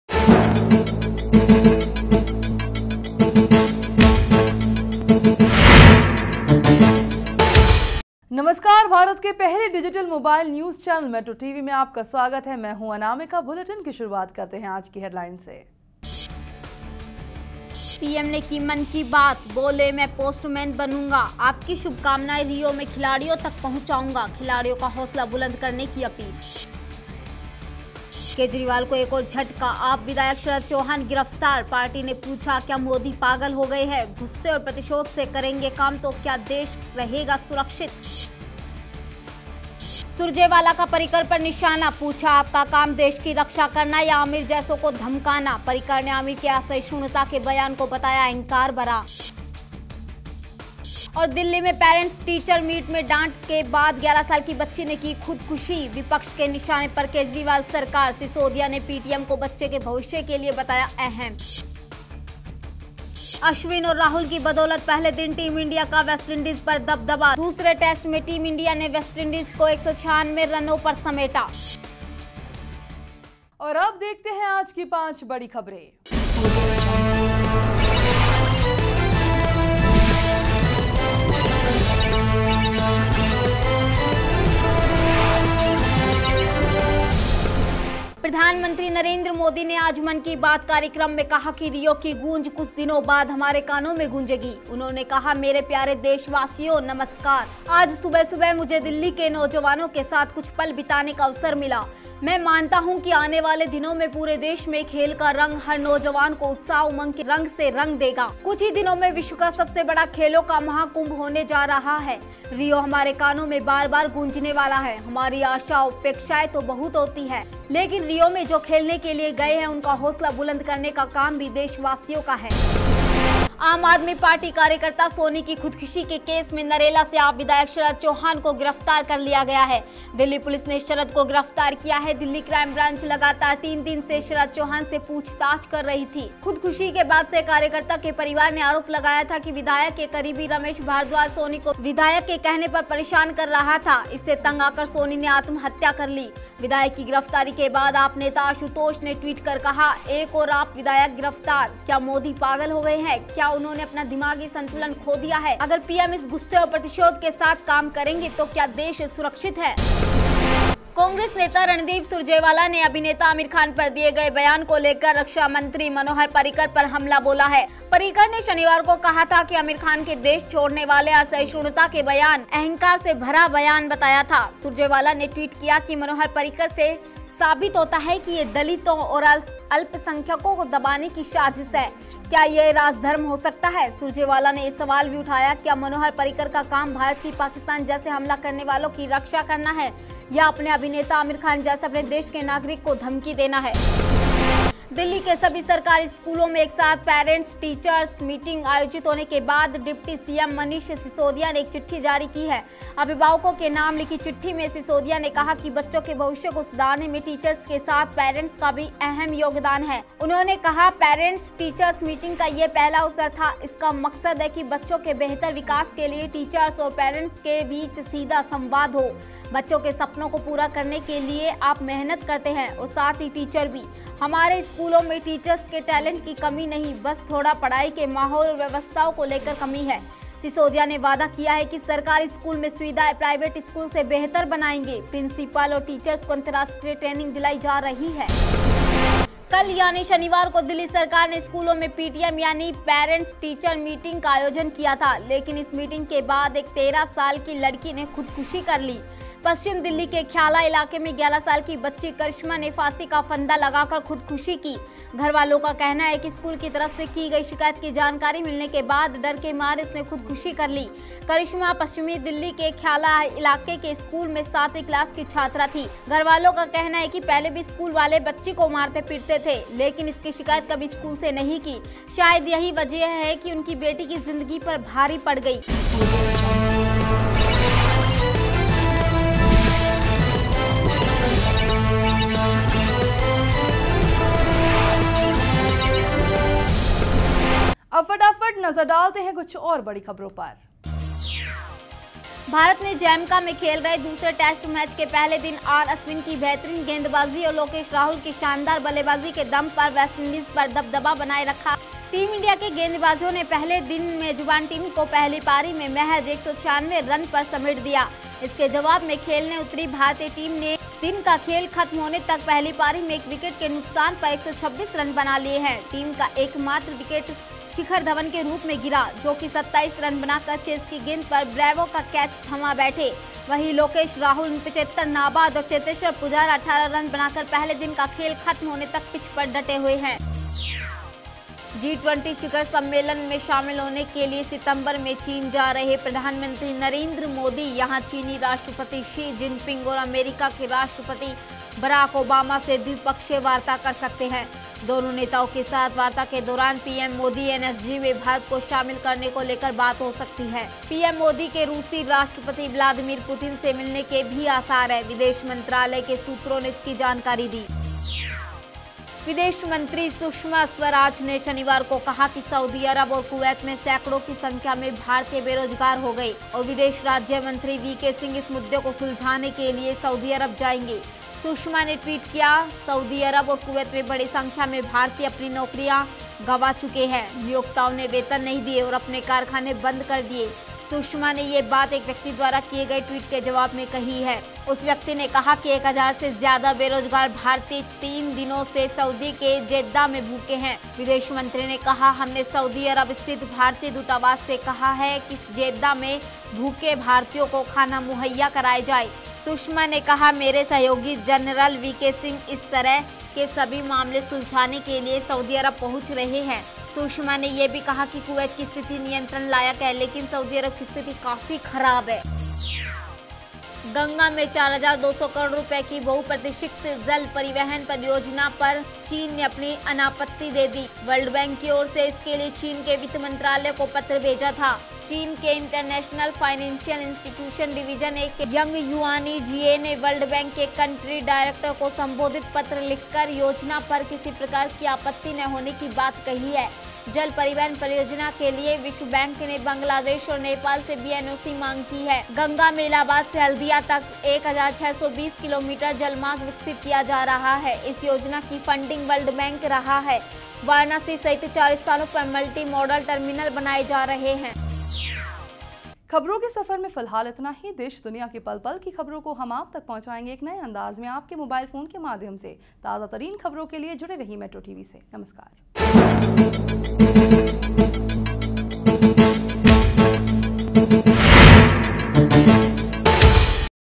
AAP MLA Sharad Chauhan Held For Woman Worker’s Suicide, AAP Party Targets PM Modi
Aam Aadmi Party legislator Sharad Chauhan and seven others were arrested on Saturday night in connection with the alleged suicide of a woman party worker. The volunteer accused Ramesh Bhardwaj, believed to be a close associate of Narela MLA Chauhan, of sexual harassment and committed suicide about two weeks ago. Bhardwaj, the prime suspect in the case, was arrested from Sonepat in Haryana. Chauhan had earlier been questioned about his links with Bhardwaj and two other alleged AAP workers, Amit Kumar and Rajini Kanth, who were accused by the woman of forcing her to withdraw her complaint against Bhardwaj. After Chauhan’s arrest, AAP’s Ashutosh hit out at the Centre and alleged vengeance. “Another AAP MLA arrested.Has Modi gone mad?Has he lost his mental balance? If PM acts with such anger/vengeance then is country safe!” he tweeted.